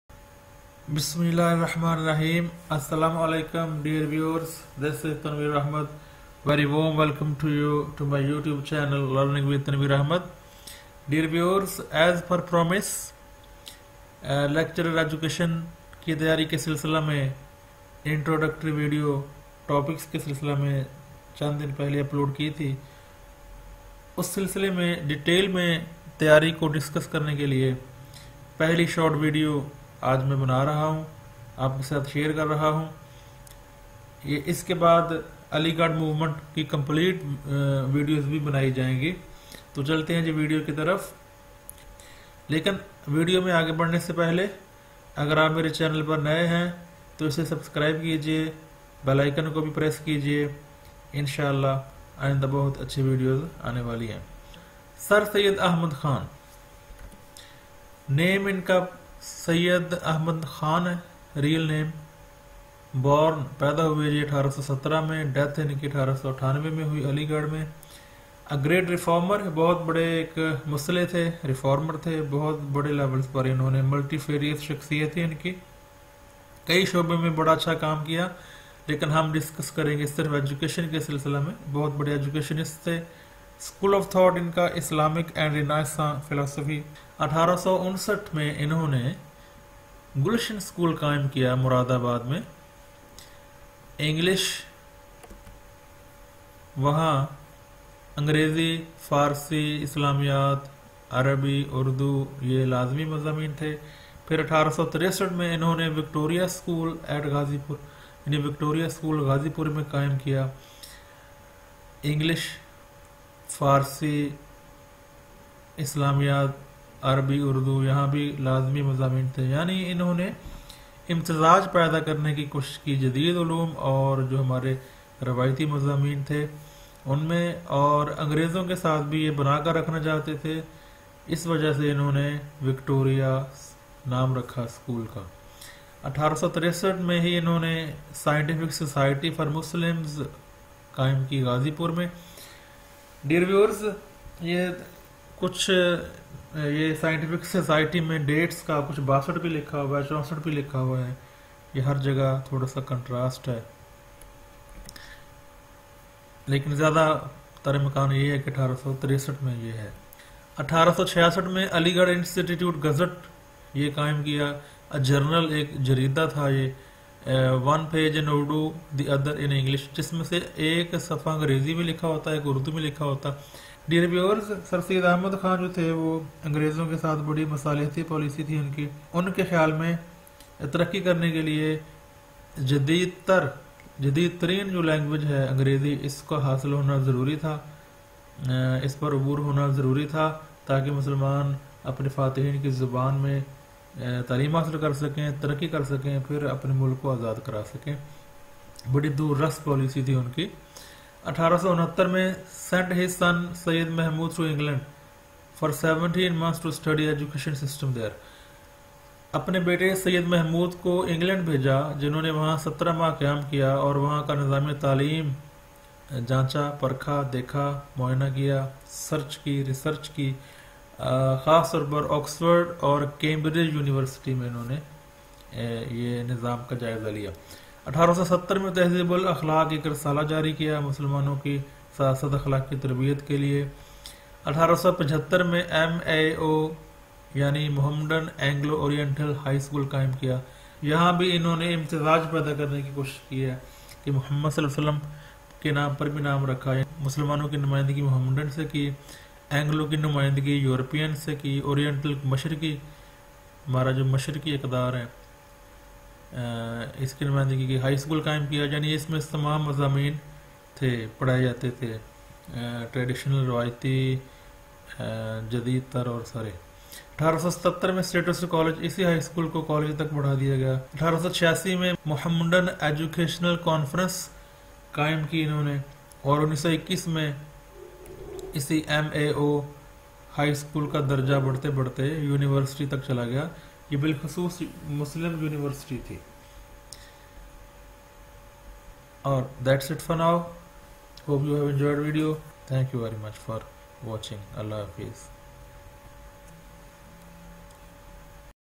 Sir Syyed Ahmed Khan||Educational services||Brief intro
For the preparation of lecturer Education SS Education Headmaster and Headmistress, the first video on the services of Sir Syed Ahmed Khan has been presented.